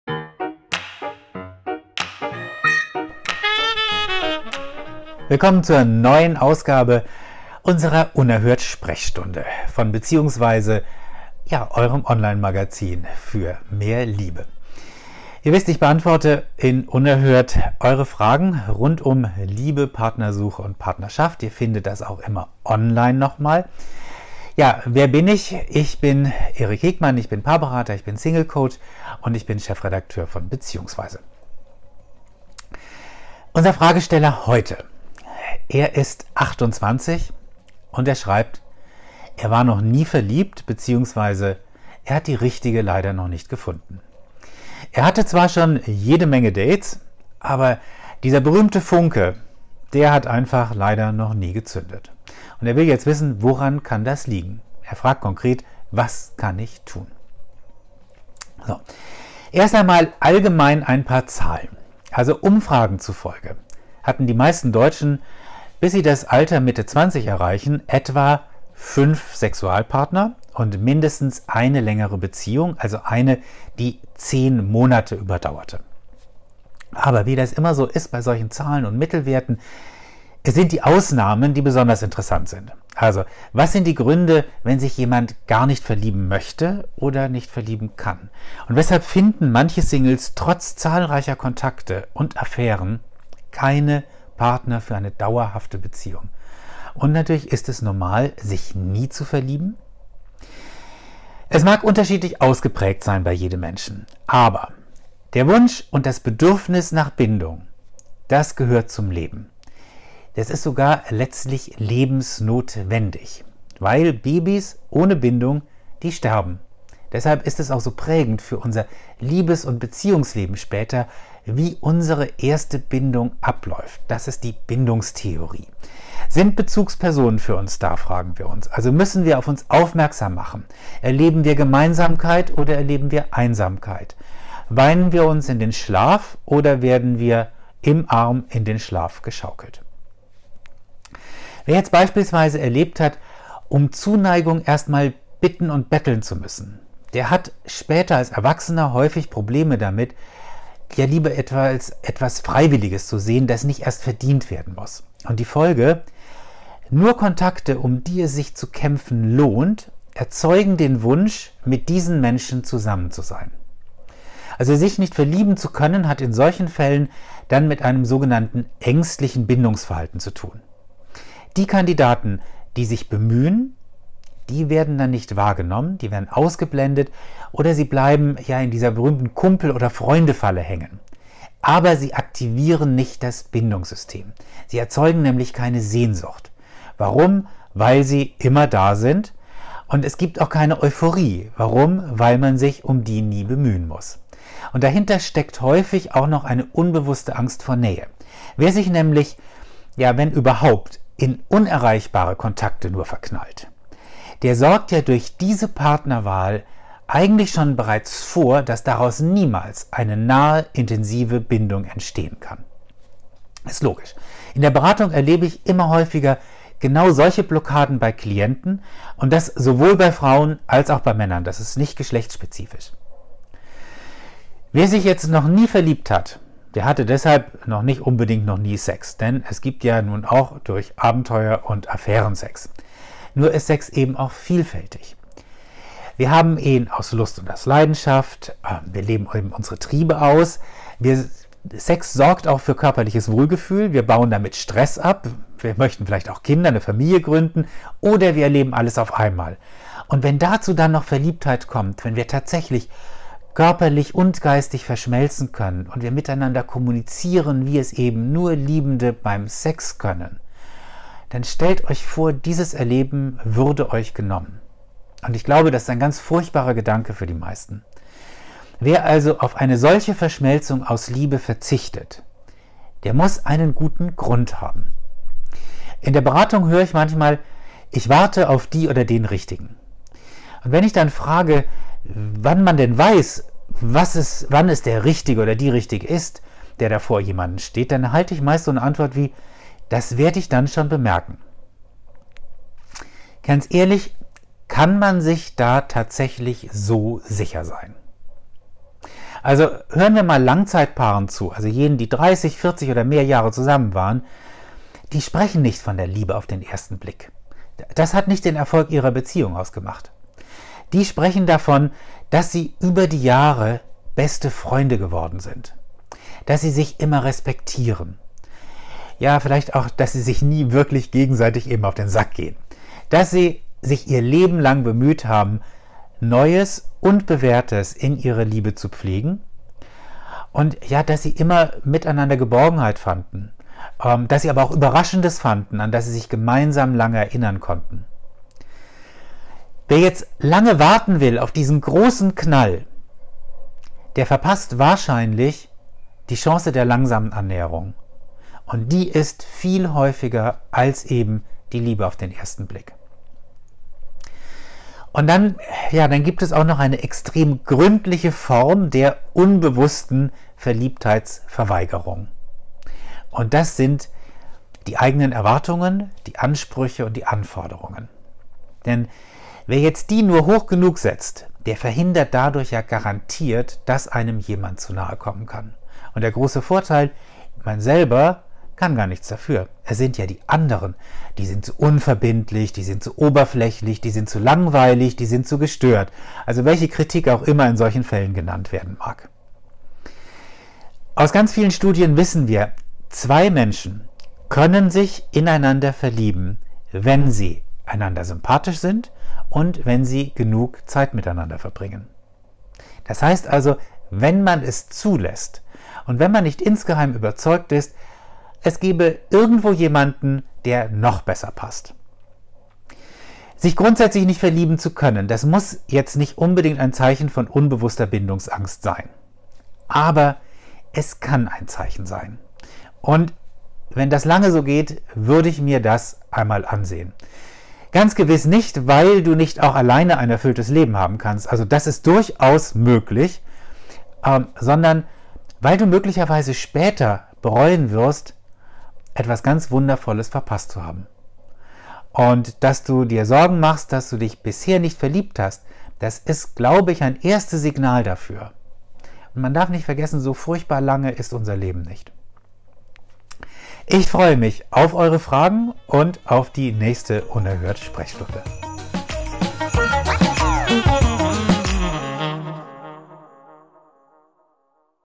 Ich war noch nie richtig verliebt - unerhört Sprechstunde Folge 10
Die "unerhört Sprechstunde" ist das neue Video-Format unseres beliebten Beziehungs- und Liebesratgebers "unerhört ehrlich". Unser Chefredakteur und Paarberater Eric Hegmann beantwortet die Fragen unserer Leser*innen.

FOLGE 10: "Ich war noch nie richtig verliebt"